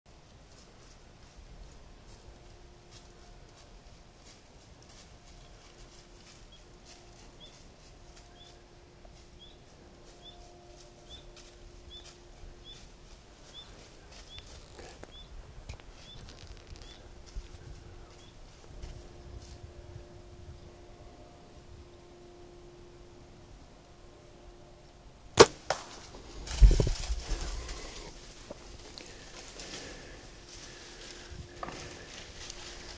Didn't Go 20!
If the arrow didn't kill him the tree sure did!